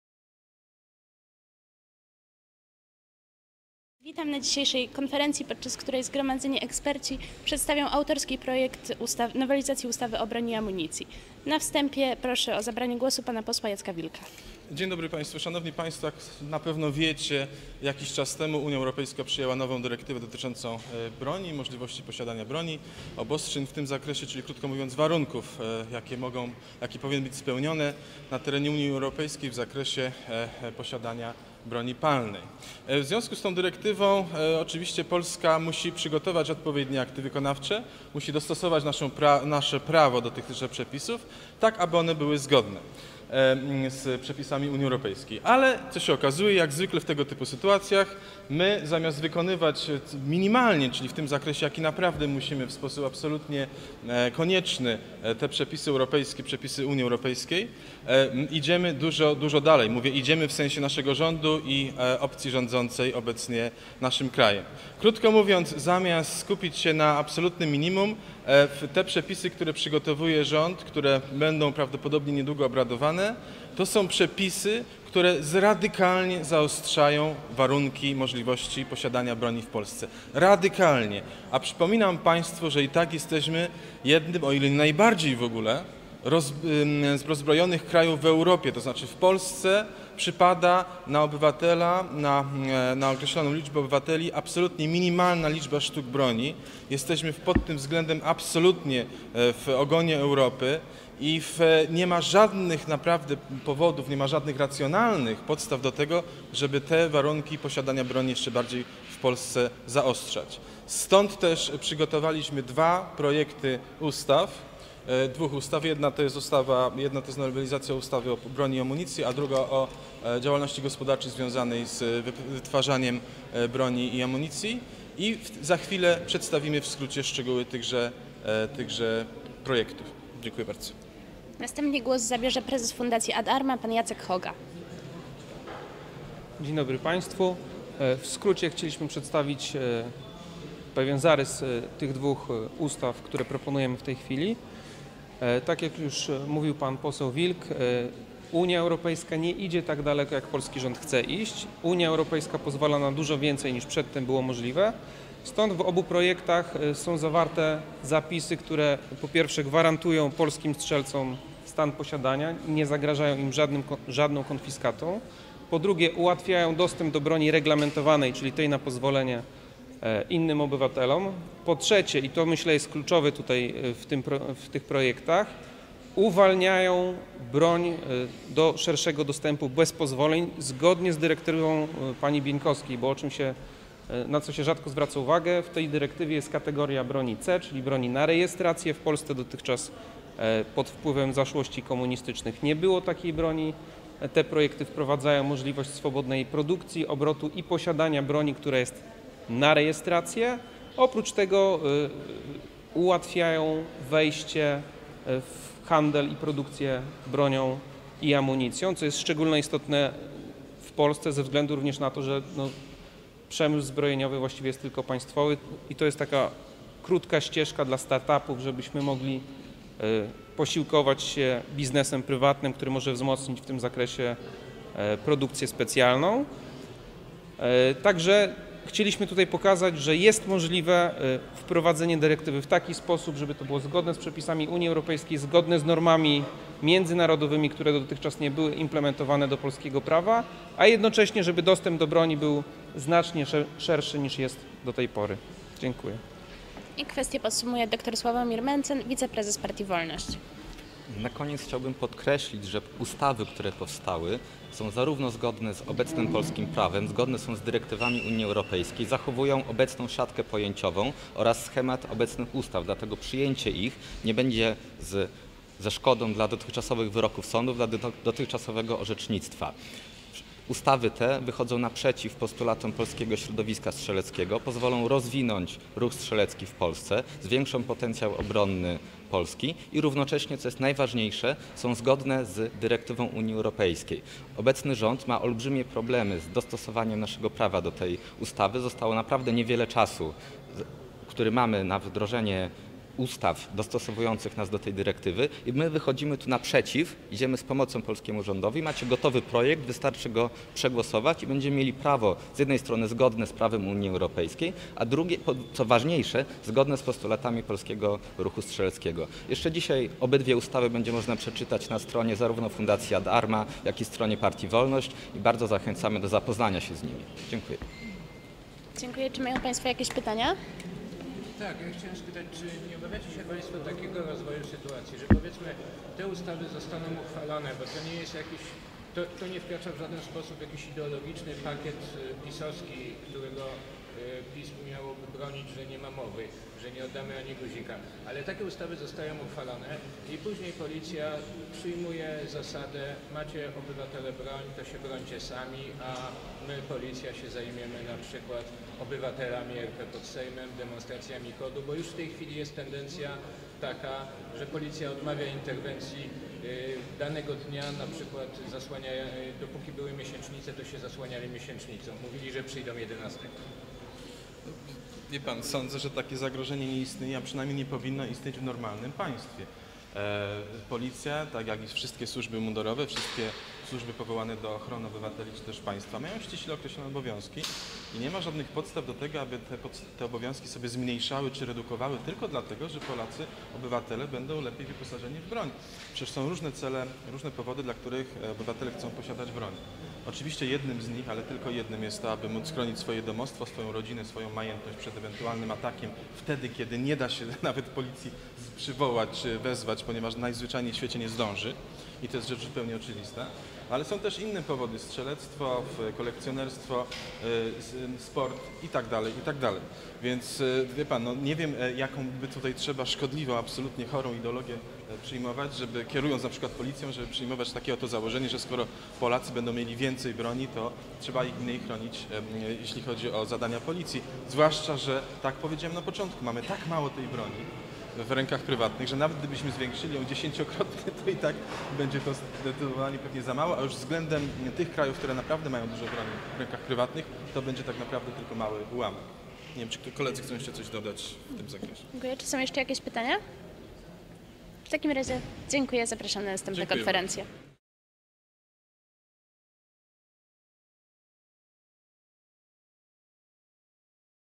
Projekt ustawy o broni i amunicji - Jacek Wilk, dr Sławomir Mentzen, Jacek Hoga
Jacek Wilk - poseł na Sejm RP
dr Sławomir Mentzen - wiceprezes partii Wolność
Jacek Hoga - prezes fundacji Ad Arma
Konferencja prasowa w Sejmie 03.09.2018
Odwiedź również: